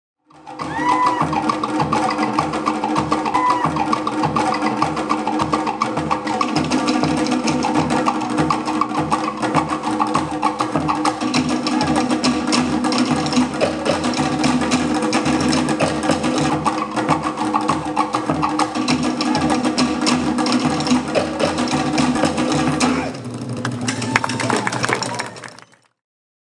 Tahiti dance
Short time lapse on the jump: no tripod, seat on the floor, my canon with a 70/200.